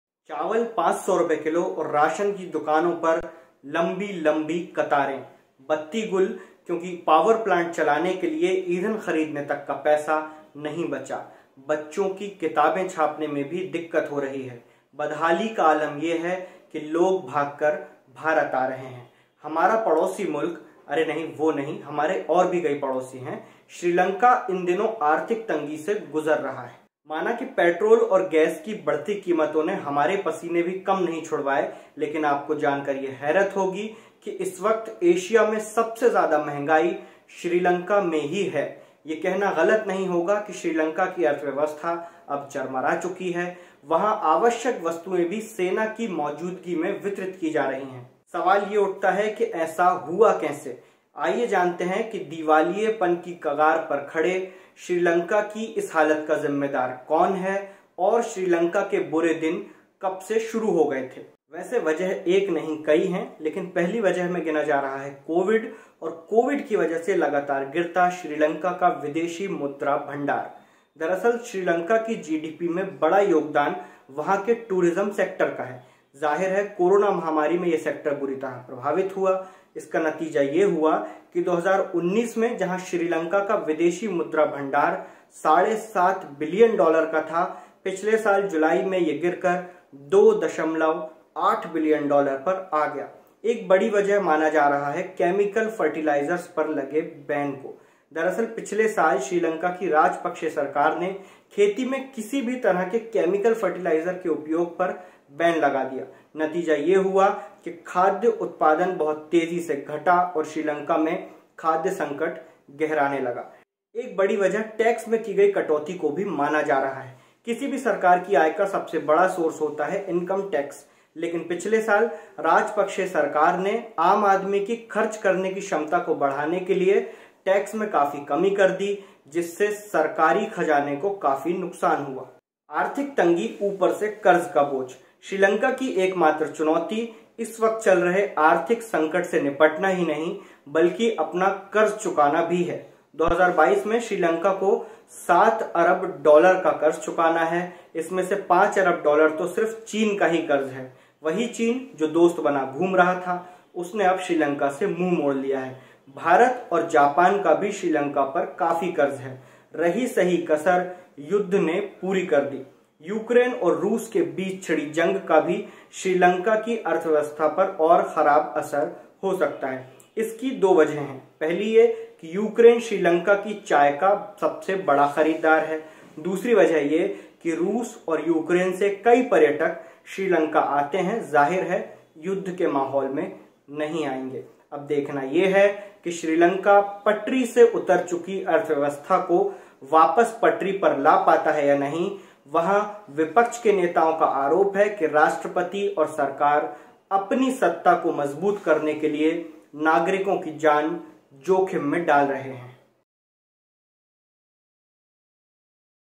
Sri Lanka Crisis: श्री लंका की अर्थव्यवस्था बुरी तरह से चरमरा गई है, आखिर ऐसा हुआ कैसे? |Quint Hindi
Sri Lanka की अर्थव्यवस्था बुरी तरह से चरमरा गई है, आवश्यक वस्तुएं सेना की मौजूदगी में वितरित की जा रही हैं, चारों तरफ हाहाकार मचा हुआ है.

अब सवाल ये उठता है कि आखिर ऐसा हुआ कैसे? श्रीलंका की इस हालत की वजह क्या है? आइए जानते हैं दिवालिया होने की कगार पर खड़े श्रीलंका की ऐसी स्थिति क्यों हुई? कब से यहां के हालात खराब होने शुरु हुए? और क्यों लगाना पड़ा यहां आर्थिक आपातकाल. कोई एक कारण नहीं है बल्कि कई ऐसी वजहे है. आइए समझते हैं